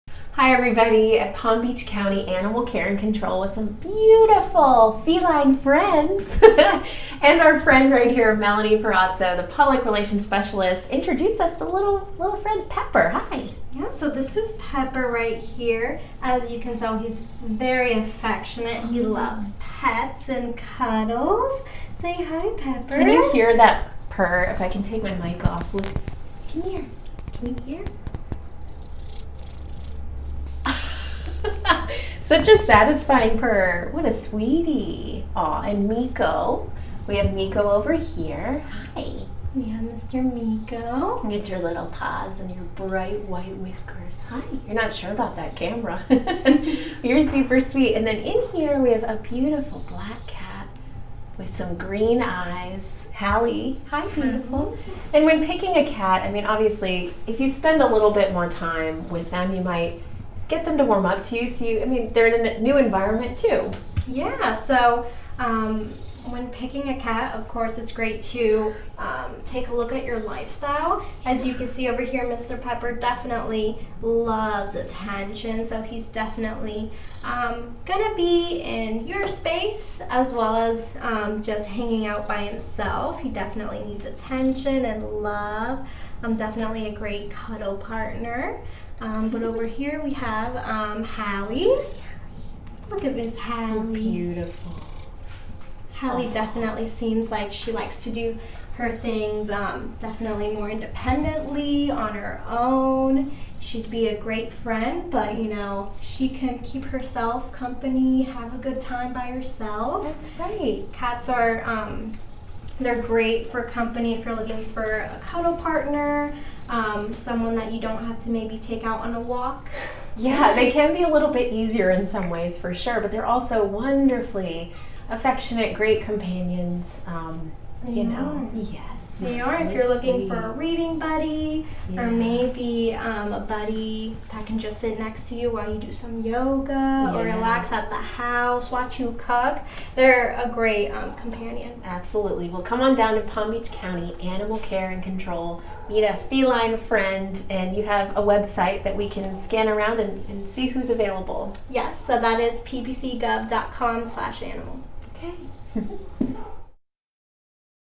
Cats up for adoption in Palm Beach County, hoping for fur-ever home
There are several cats up for adoption at Palm Beach County Animal Care and Control. We met three of them in hopes of finding them each a fur-ever home.